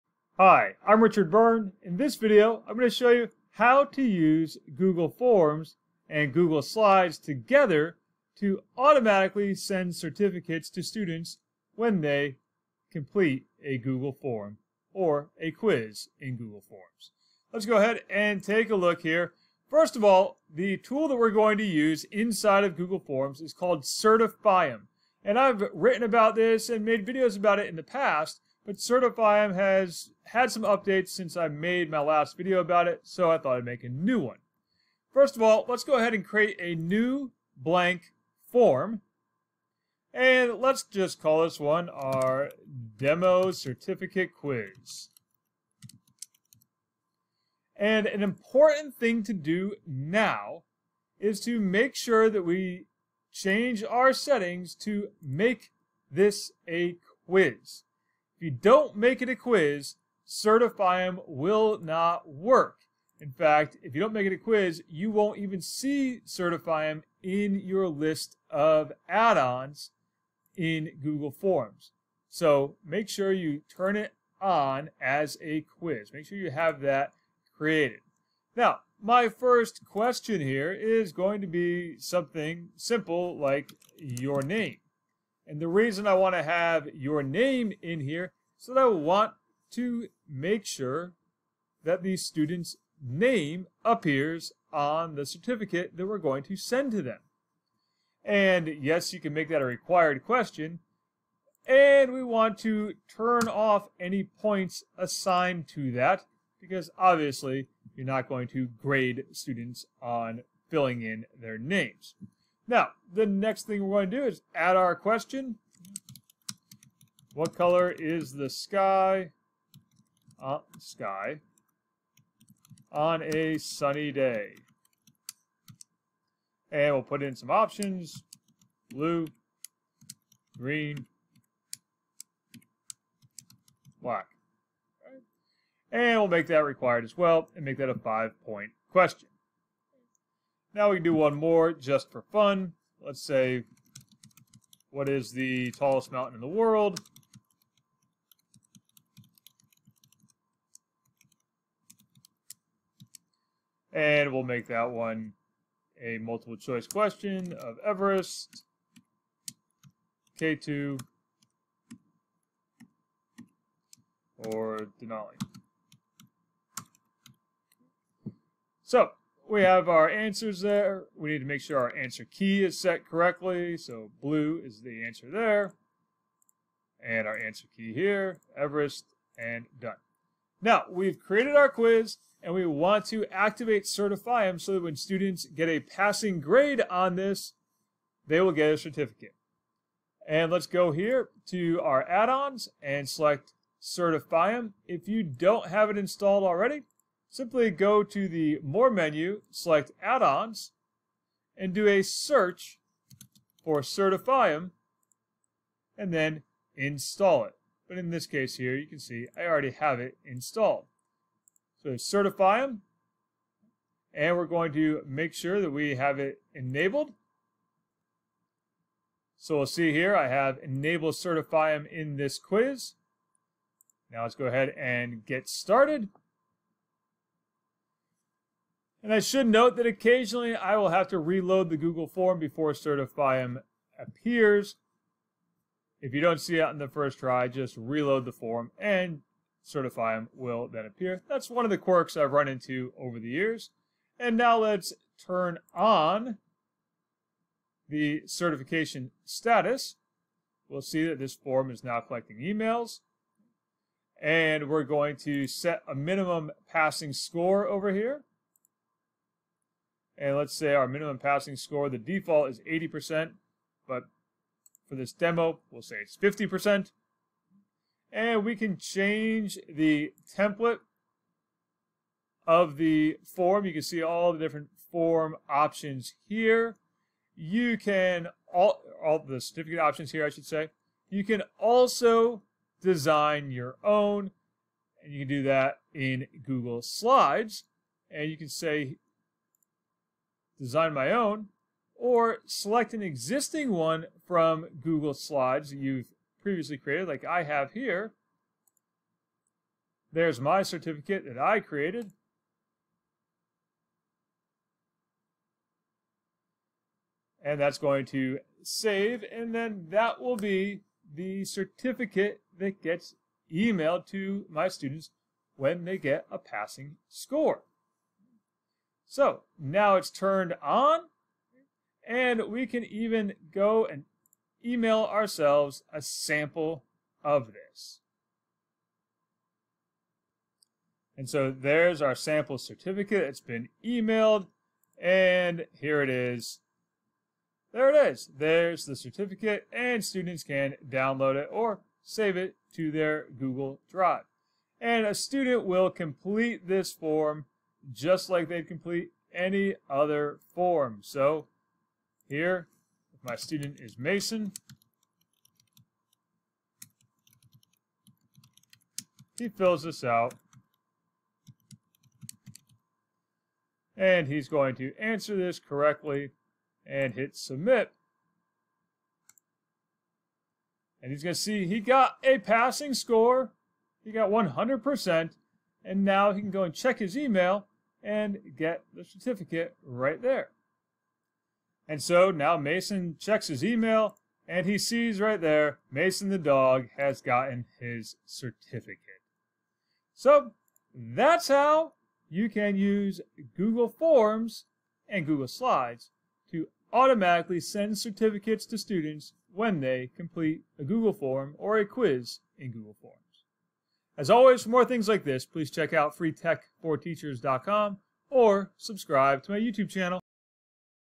How to Automatically Send Certificates for Passing Quizzes in Google Forms
Certify'em is a great Google Forms add-on that makes it easy to automatically send certificates to students when they get a passing score on a quiz. Watch this updated video to see how it works.
🏫 Take a course with me: 🏫